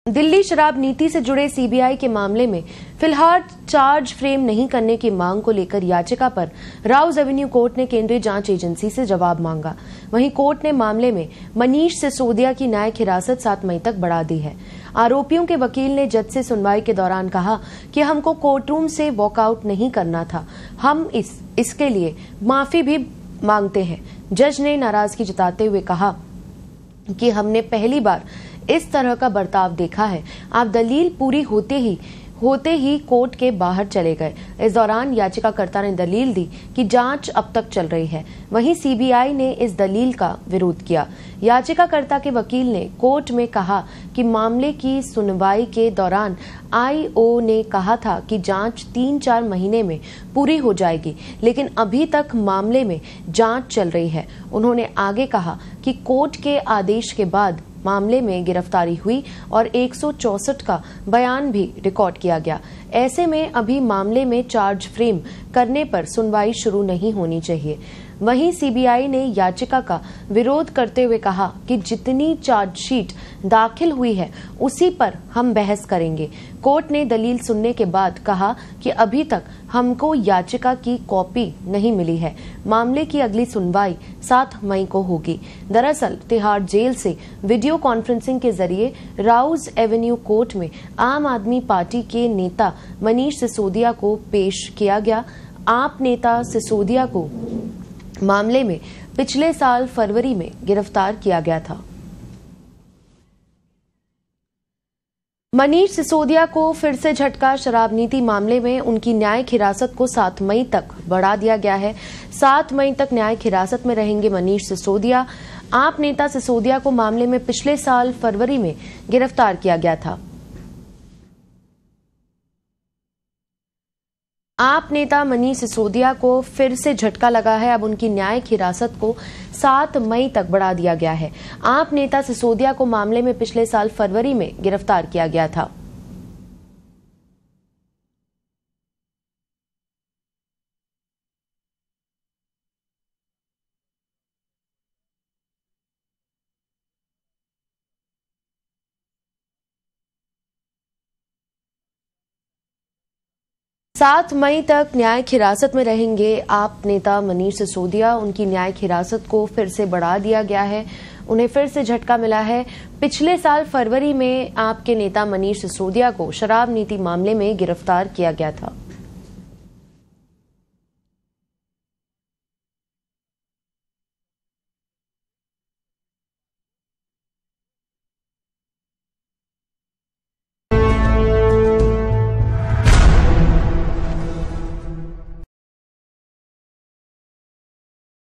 Delhi Liquor Policy Scam: Manish Sisodia की न्यायिक हिरासत 7 मई तक बढ़ी
Delhi Liquor Policy Scam: Manish Sisodia की न्यायिक हिरासत 7 मई तक बढ़ी । Delhi Liquor Policy Scam । Manish Sisodia । Aam Admi Party । Rouse Avenue Court

(Delhi Liqour Policy)दिल्ली शराब नीती मामला...(Manish Sisodia)मनीष सिसोदिया को झटका... न्यायिक हिरासत 7 मई तक बढ़ी... (Rouse Avenue Court)कोर्ट ने CBI से मांगा जवाब...